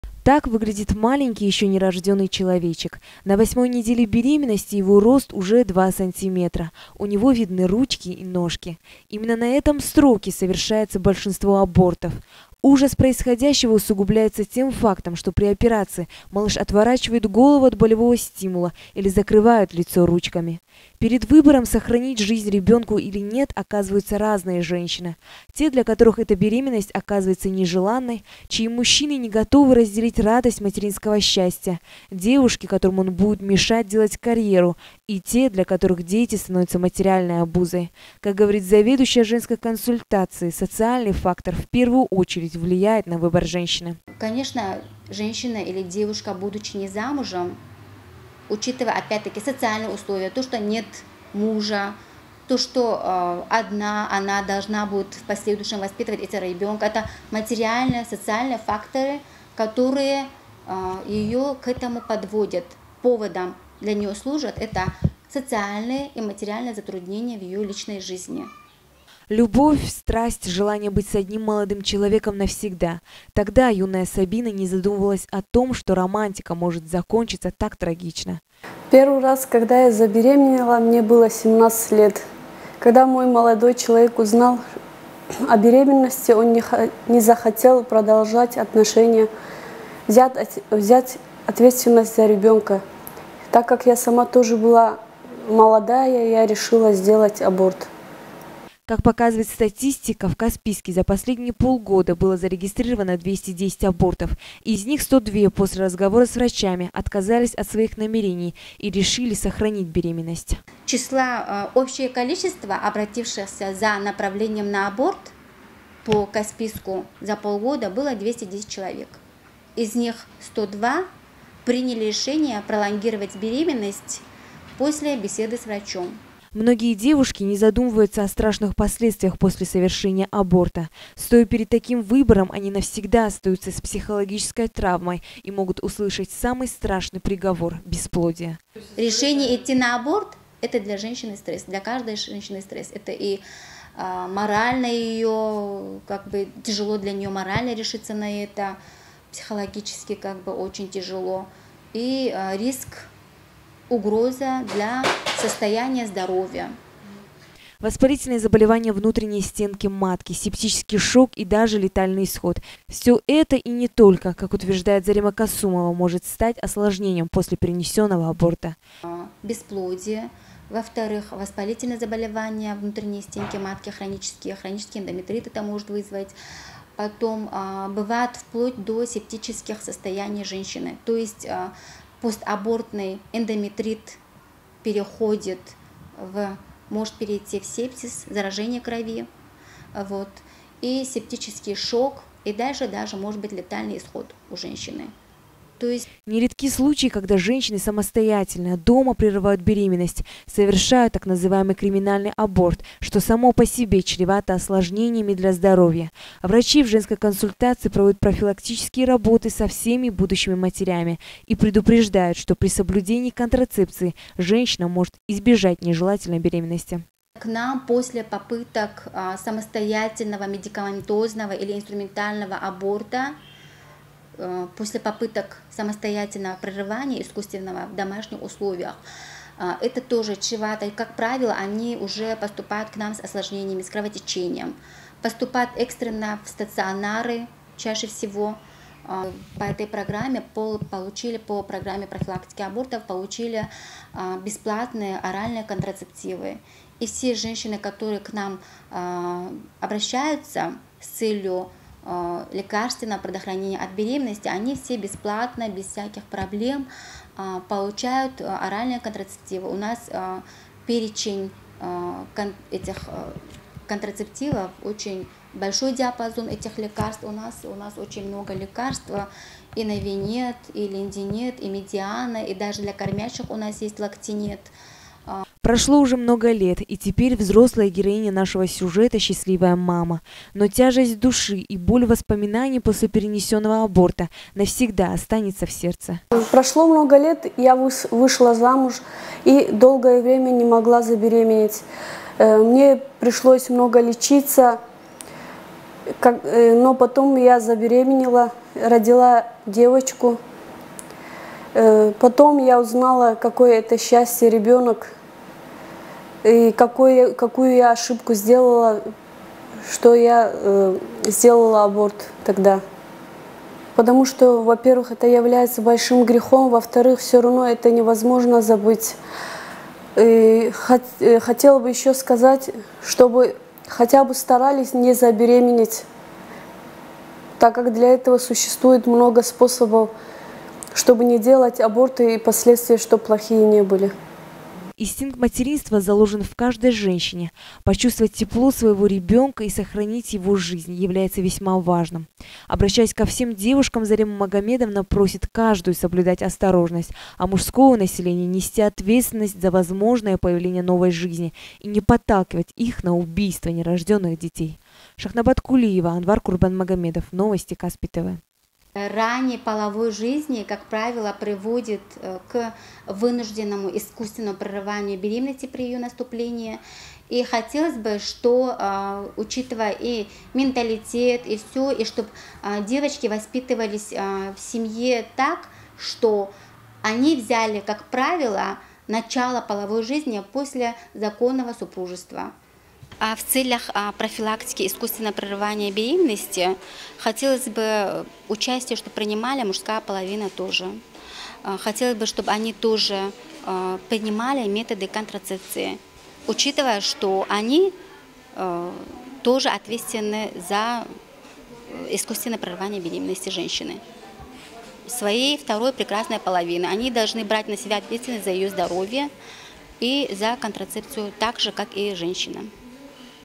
Добровольное убийство не рожденных детей.
Анонс: Добровольное убийство не рожденных детей.

Так выглядит маленький еще не рожденный человечек, на 8 недели беременности его рост уже 2 см. У него  видны, ручки и ножки, Именно на этом сроке совершается большинство абортов.  Ужас происходящего усугубляется тем фактом, что при операции малыш  отворачивает голову от болевого стимула или закрывает лицо ручками. Перед выбором, сохранить жизнь ребенку или нет оказываются разные женщины, те для которых эта  беременность оказывается не желанной, чьи мужчины не готовы разделить радость материнского счастья,  девушки, которым он будет мешать делать карьеру и те для  которых дети становятся материальной  обузой. Как говорит заведующая ЖК социальный фактор в первую очередь влияет на выбор женщины.
Любовь, страсть желание быть с  одним молодым человеком навсегда, тогда юная Сабина не задумывалась о том. Что романтика может закончится так трагично.
Как показывает статистика за последние полгода в Каспийске  было зарегистрировано  210 абортов, из них 100 после разговора с врачами отказались от своих намерений и решили сохранить беременность.

Многие девушки не задумываются о страшных последствиях после совершения аборта. Стоя  перед таким  выбором, они навсегда остаются с  психологической травмой. И могут  услышать самый страшный приговор бесплодие.

Воспалительное заболевание внутренней стенки матки, септический шок и даже летальный исход все это и не только как,  утверждает Зарема Касумова может стать осложнением после перенесенного аборта.

Не редки случаи когда женщины самостоятельно дома прерывают беременность, совершают так называемый криминальный аборт , что само по себе чревато осложнениями для здоровья.  Врачи в ЖК  проводят профилактические работы со всеми будущими матерями, и предупреждают, что при соблюдении  контрацепции, женщина может избежать не желательной беременности.

Прошло  уже много лет и теперь взрослая героиня нашего сюжета счастливая мама, но  тяжесть души и боль воспоминаний после перенесенного аборта навсегда останется  в сердце.

Инстинкт материнства заложен в каждой женщине, почувствовать тепло своего ребенка, и сохранить его жизнь является весьма важным.   Обращаясь  ко всем девушкам Зарема Магомедовна  просит каждую соблюдать осторожность,   а  мужское население нести ответственность за возможное появление новой жизни,  и не подталкивать их    на убийство не рожденных детей.